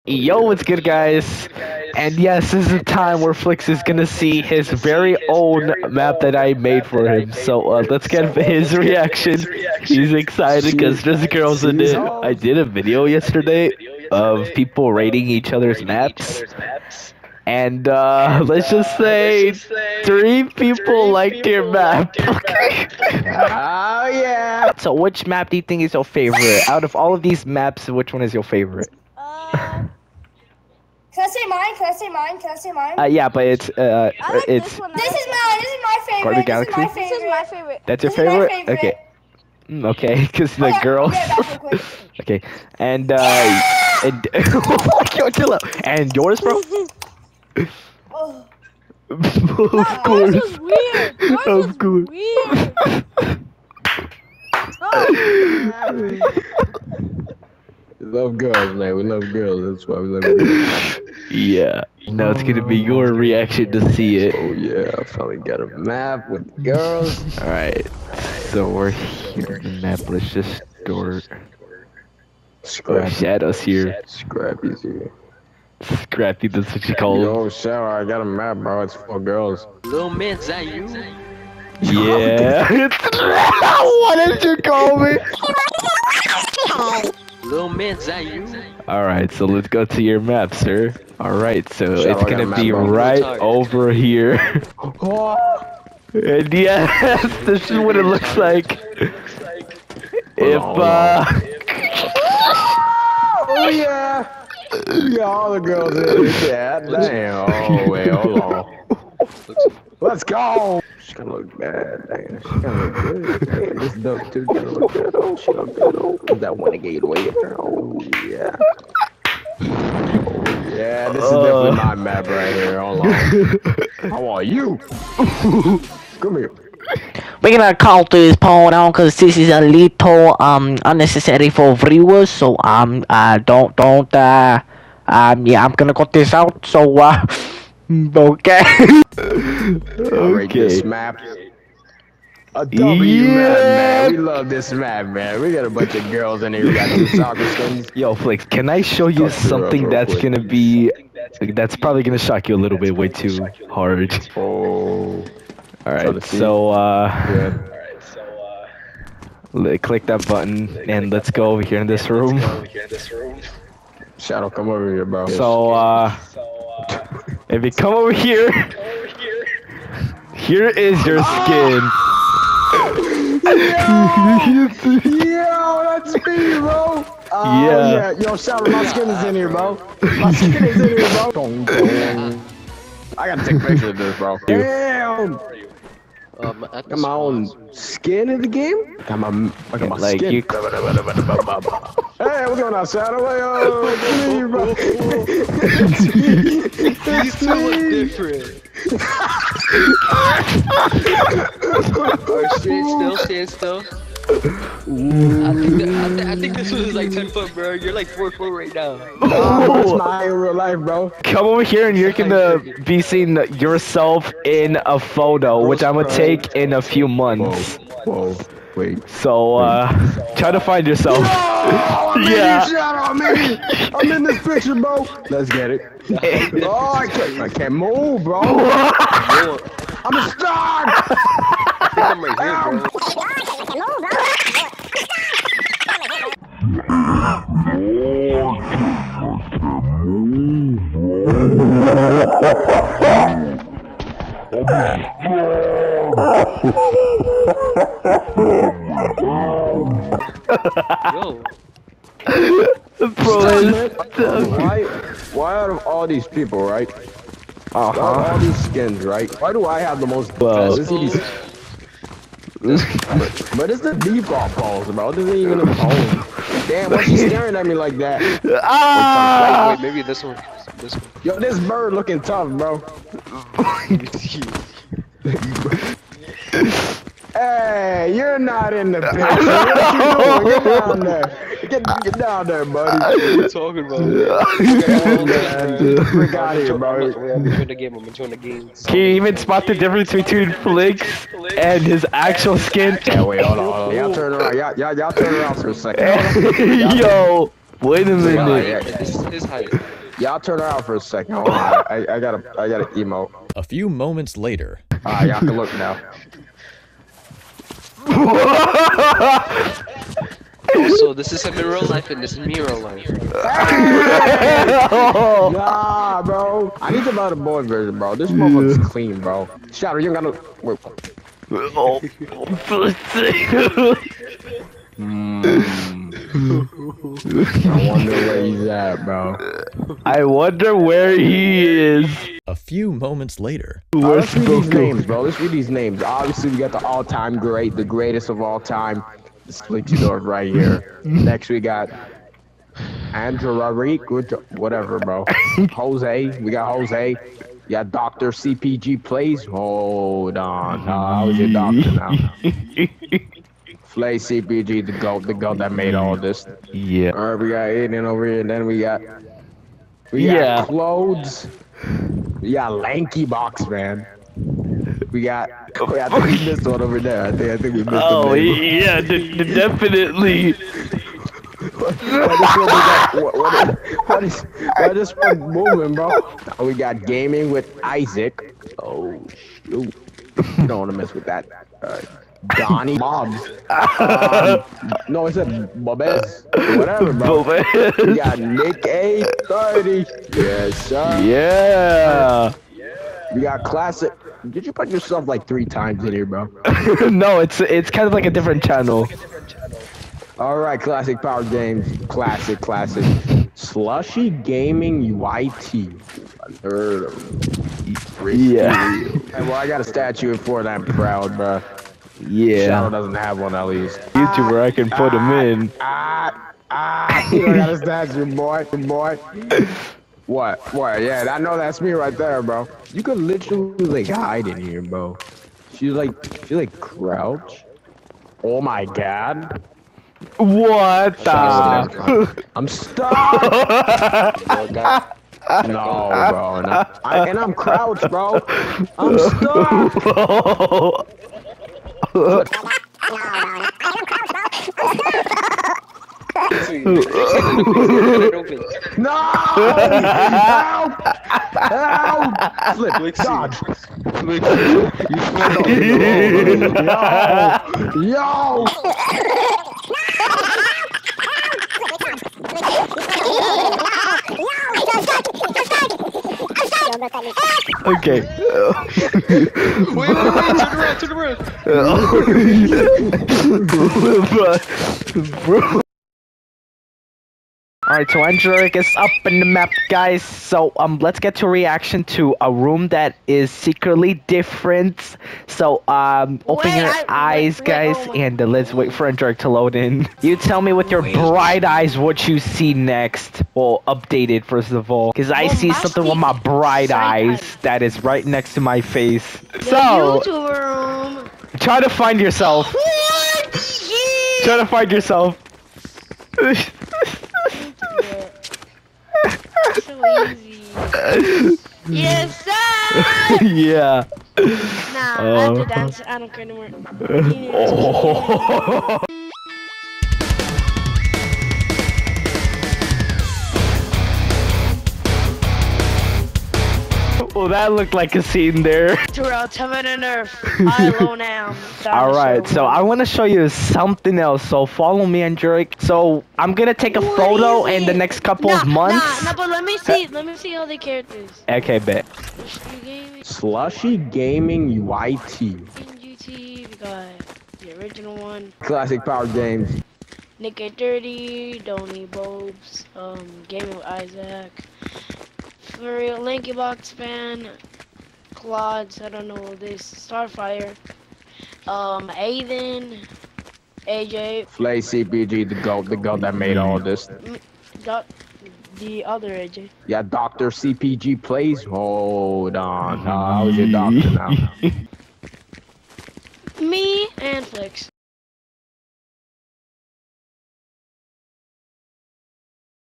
They React To Their Own YouTube Skin and Their maps in my RVB!
Their reaction to their own skin in the youtube room and their own skin into a picture.
0:00 Intro
0:09 FLIXYDORF
0:24 Real Footage
0:56 FLIXYDORF's REACTION
2:27 FLIXYDORF's YouTube Skin
3:02 I gotta take of this bro
3:04 FLIXYDORFS REACTION TO YouTube Skin
5:34 FLIXYDORF Reads The YouTube Viewers And YouTubers
6:49 He's Proud Of His Skin!
7:02 FLIXYDORF Gets Stuck
7:44 I get stuck
7:55 Angerick's Turn
8:59 Angerick Reads the YouTube Viewers and YouTubers
9:37 Outro